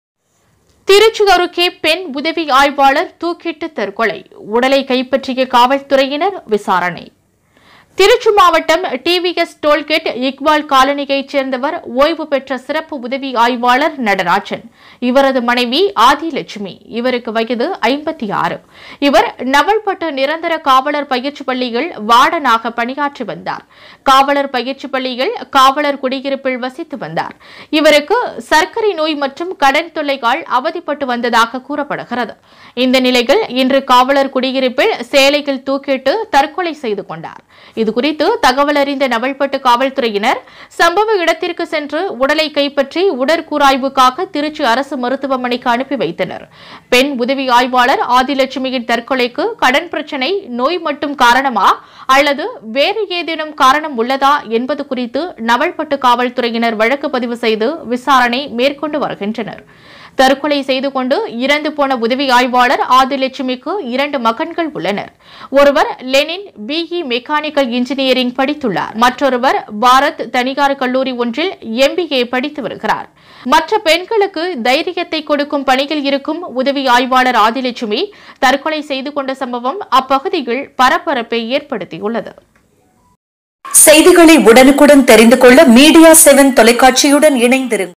திருச்சி அருகே பெண் உதவி ஆய்வாளர் தூக்கிட்டு தற்கொலை...
திருச்சி அருகே  பெண் உதவி ஆய்வாளர் தூக்கு மாட்டி தற்கொலை - உடலை கைப்பற்றிய காவல்துறையினர் விசாரணை.

திருச்சி மாவட்டம், டிவிஎஸ் டோல்கேட் இக்பால் காலனியை சேர்ந்தவர் ஓய்வு பெற்ற சிறப்பு உதவி ஆய்வாளர் நடராஜன்.
இவரது மனைவி ஆதிலட்சுமி(56). நவல்பட்டு நிரந்தர காவலர் பயிற்சிப் பள்ளியில் வார்டனாக பணியாற்றி வந்தார். காவலர் பயிற்சிப் பள்ளியில் காவலர் குடியிருப்பில் வசித்து வந்தார். இவருக்கு  சர்க்கரை நோய் மற்றும் கடன் தொல்லையால் அவதிப்பட்டு வந்ததாக கூறப்படுகிறது.
இந்த நிலையில் இன்று காவலர் குடியிருப்பில் சேலையில் தூக்கு மாட்டி தற்கொலை செய்து கொண்டார்.
இதுகுறித்து தகவலறிந்த நவல்பட்டு காவல்துறையினர் சம்பவ இடத்திற்கு சென்று உடலை கைப்பற்றி உடற்கூறு ஆய்வுக்காக திருச்சி அரசு மருத்துவமனைக்கு அனுப்பி வைத்தனர். பெண் உதவி ஆய்வாளர் ஆதிலட்சுமியின் தற்கொலைக்கு கடன் பிரச்சனை நோய் மட்டும் காரணமா அல்லது வேறு ஏதேனும் காரணம் உள்ளதா என்பது குறித்து
நவல்பட்டு காவல்துறையினர் வழக்கு பதிவு செய்து  விசாரணை மேற்கொண்டு வருகின்றனர்.

தற்கொலை செய்துகொண்டு இறந்து போன உதவி ஆய்வாளர் ஆதிலட்சுமிக்கு
இரண்டு மகன்கள் உள்ளனர்.
ஒருவர் லெனின் BE மெக்கானிக்கல் இன்ஜினியரிங் படித்துள்ளார்.
மற்றொருவர் பாரத், தனியார்  கல்லூரி ஒன்றில் எம்பிஏ படித்து வருகிறார்.

மற்ற பெண்களுக்கு தைரியத்தை கொடுக்கும் பணியில் இருக்கும் உதவி ஆய்வாளர் ஆதிலட்சுமி தற்கொலை செய்துக்கொண்ட சம்பவம் அப்பகுதியில் பரபரப்பை ஏற்படுத்தி உள்ளது.